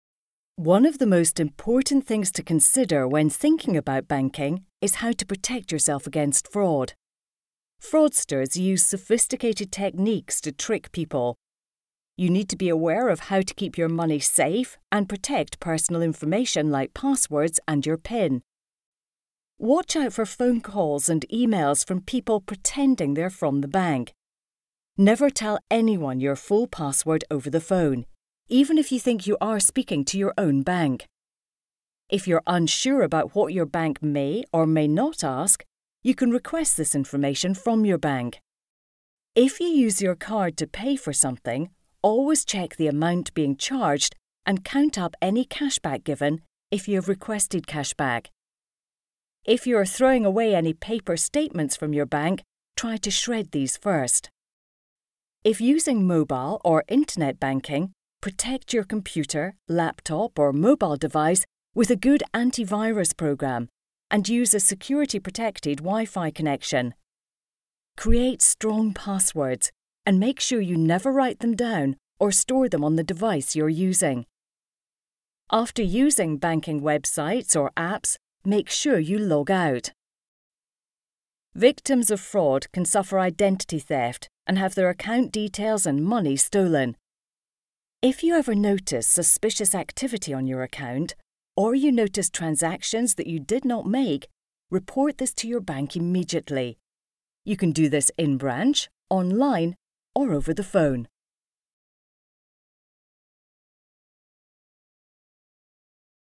Accessing the Bank 5: Security and Fraud
Thanks to generous funding from the Clydesdale Bank Spirit of the Community Award, we have been able to develop six videos to support and enable individuals with autism to manage some of the unique challenges they face when going to the bank. These short animations highlight some of the common experiences that come with a trip to the bank such as waiting in queues, speaking to different people and having to remember PIN codes. They also include advice on how to keep money safe and guard against fraud as well as highlight how an individual can transact online or over the telephone with their bank.